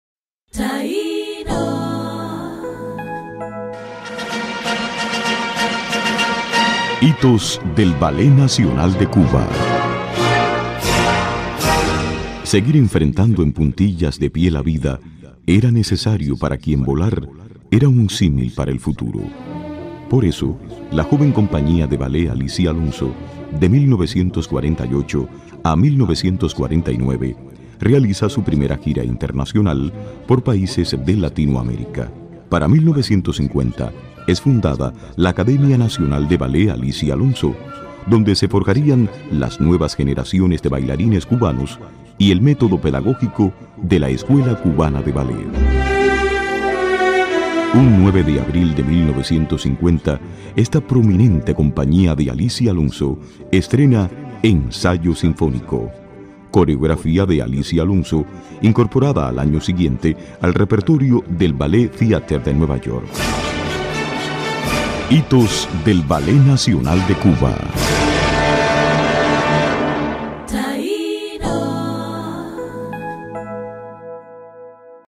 Cuba: Hitos del Ballet Nacional de Cuba (Cap1)
El alma de las cosas hace que sean tangibles, por eso el 28 de octubre de 1948, es fundado con el nombre de Ballet Alicia Alonso, el actual Ballet Nacional de Cuba, primera compañía profesional de ballet en la historia de Cuba.

Dirección y Edición (Youtube): Idalberto Moltó Carrasco
Voz : Frank Abel Gómez Bernal
Dirección (Versión Radio) : Lázaro Caballero Aranzola
Guión (Versión Radio): Luis Ríos Vega